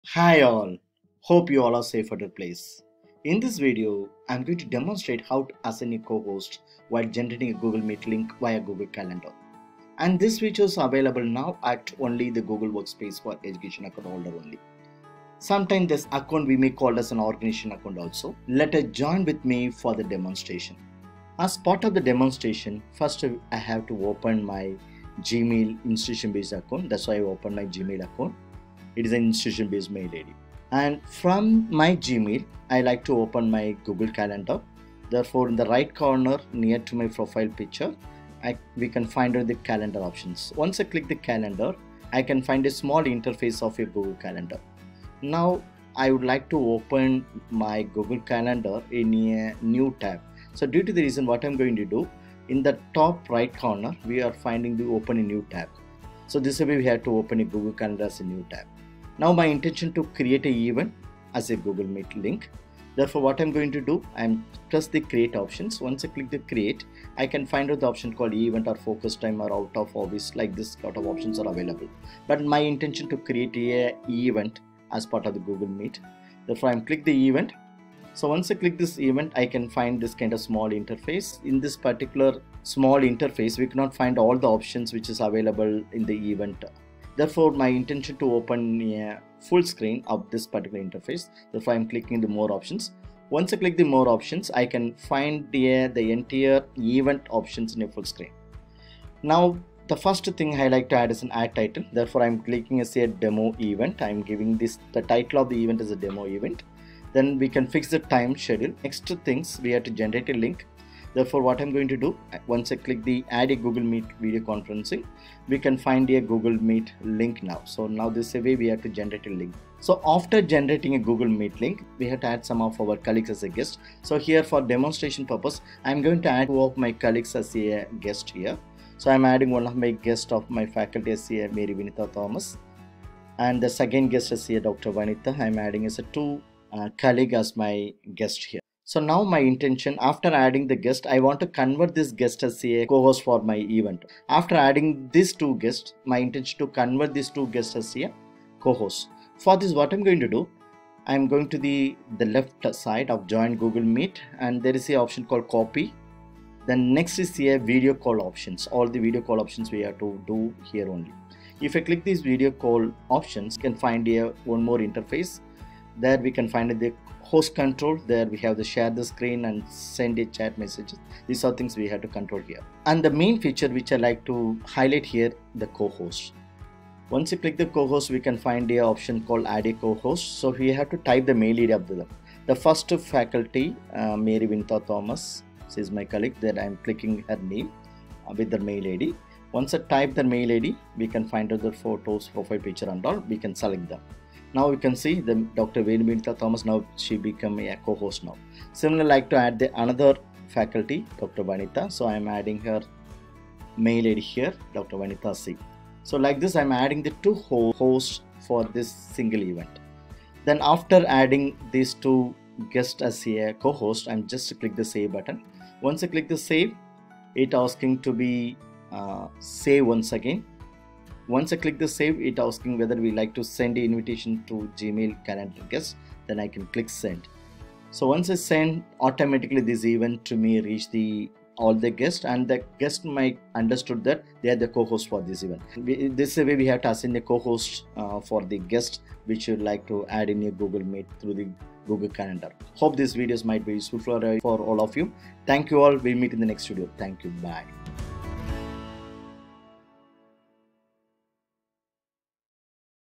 Assign a Co-host while generating a Google Meet link via Google Calendar (Workspace for Education)
How to assign a Co-host while generating a Google Meet link via Google Calendar?
Google Meet: Co-host (Google Workspace for Education / organizational Account only)

This video is a self-explanatory tutorial on how to assign a co-host while generating a google meet link via google calendar to the guest. These features are available only to the institutional-based google workspace account holders.